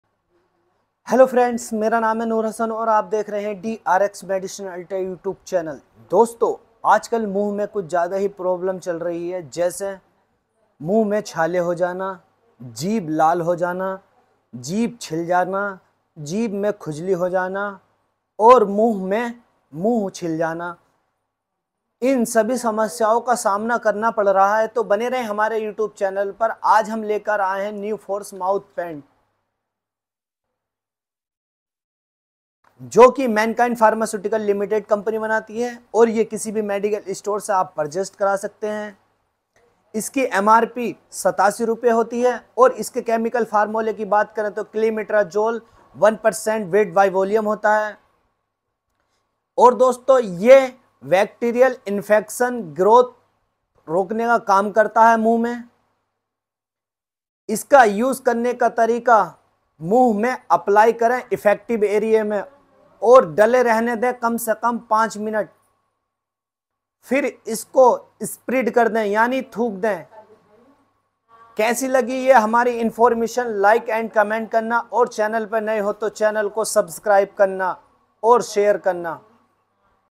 मुँह में छाले कारे सही अब Nuforce mouth paint
मुंह में छाले की समस्या से हम सभी, कभी न कभी जरूर परेशान हुए होंगे। जीभ, होंठ के पीछे या जबड़ों में होने वाले यह घाव काफी दर्दकारक और असहज करने वाले होते हैं। छाले होने पर भोजन को निगलना और पानी पीना तक काफी कठिन हो जाता है। सामान्यतौर पर माना जाता है कि पेट की गर्मी के कारण या जिन लोगों का पेट साफ नहीं होता है, उन्हें मुंह के छाले हो जाते हैं, पर अगर आपको अक्सर छाले की समस्या होती रहती है तो इसे हल्के में लेने की भूल न करें। विशेषज्ञों के मुताबिक पेट की समस्याओं के अलावा कुछ गंभीर स्थितियां भी इन छालों का कारण बन सकती हैं, जिनके बारे में जानना बहुत आवश्यक है।

आज fast time mic ke Dora baniya h video
Kaise hai awaj zaroor batana